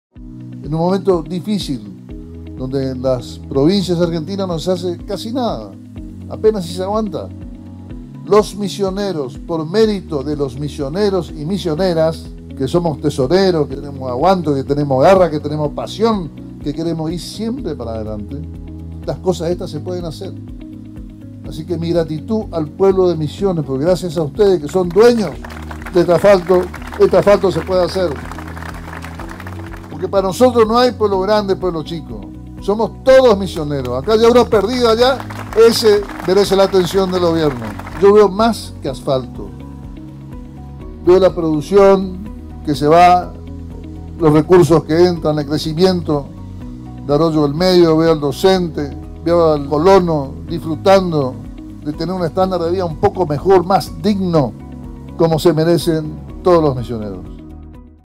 'El asfalto es producción que se mueve', dijo Passalacqua
Inauguración del el acceso a Arroyo del Medio.